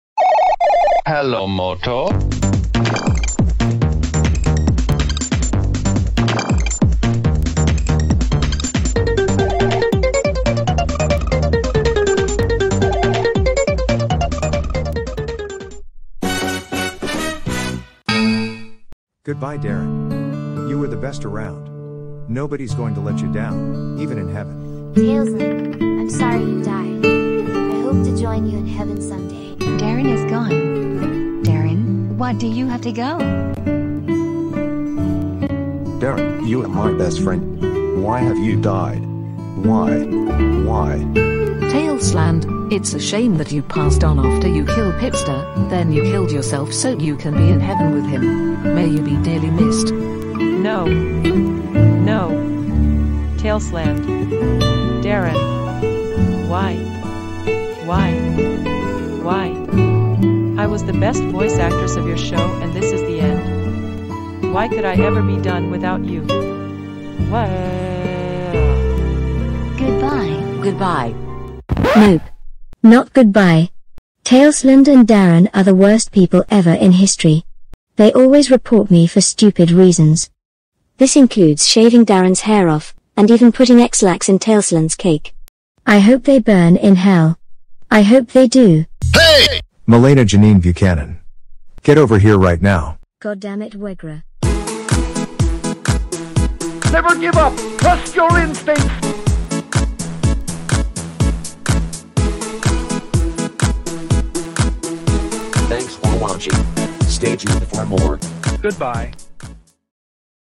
Tailslandian Gardens Funnies - Double Funeral
In this life lesson is regarding funeral etiquette. Darren and Tailsland's family and friends are there to say goodbye. But Malena, SHE DISRESPECTED! Find out what happens next and tell me why that happened.
Never give up, trust your instincts!